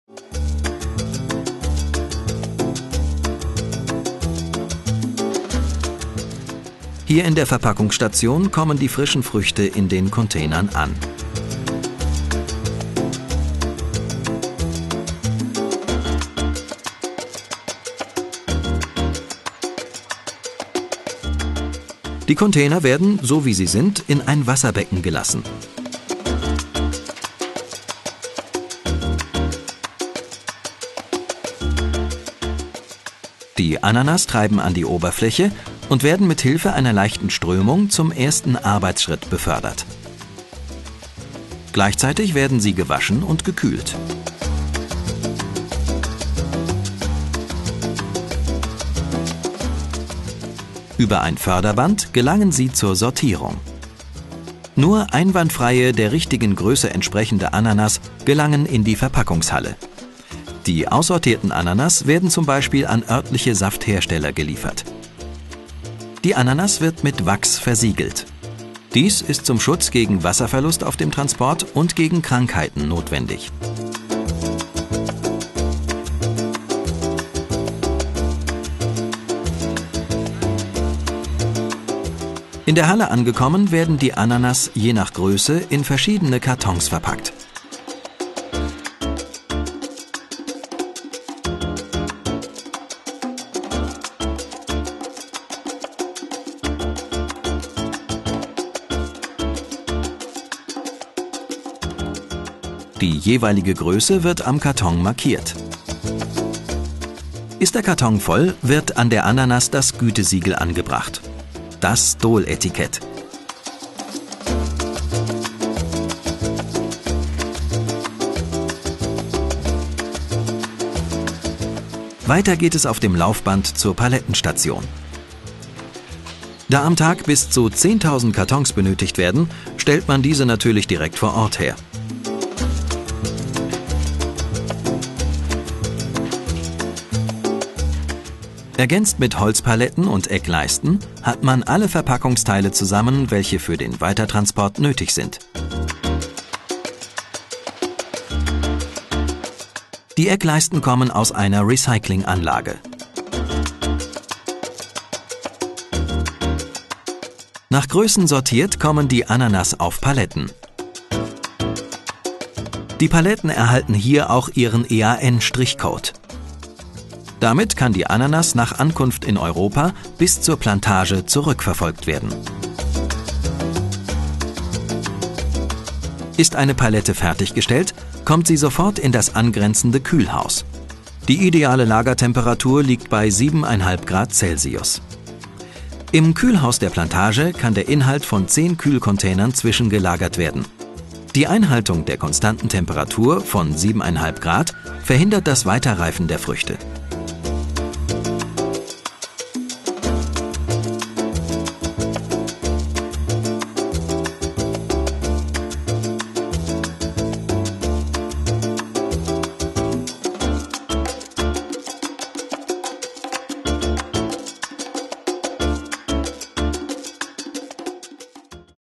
DOLE Ananas Verpackung
Dole Ananas werden gemäß der Qualitätsbestimmungen und der Größe sortiert und gepackt.